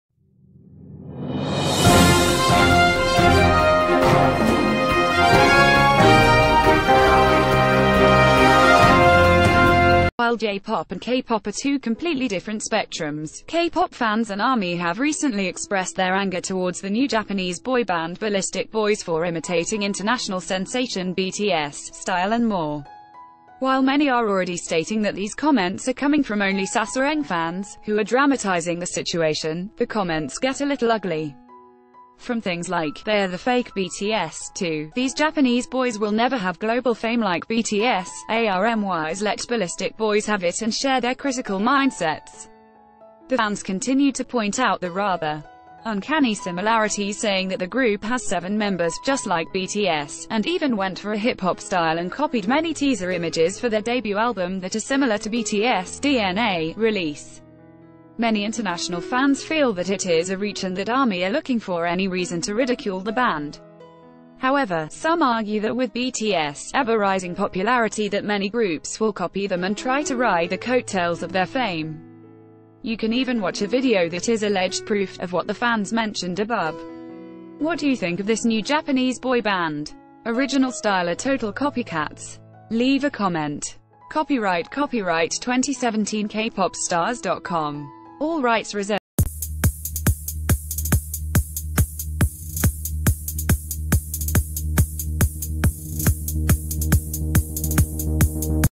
Fans Are Enraged Over This BTS 'Copy Cat' Group In Japan
While J-Pop and K-Pop are two completely different spectrums, K-Pop fans and ARMY have recently expr...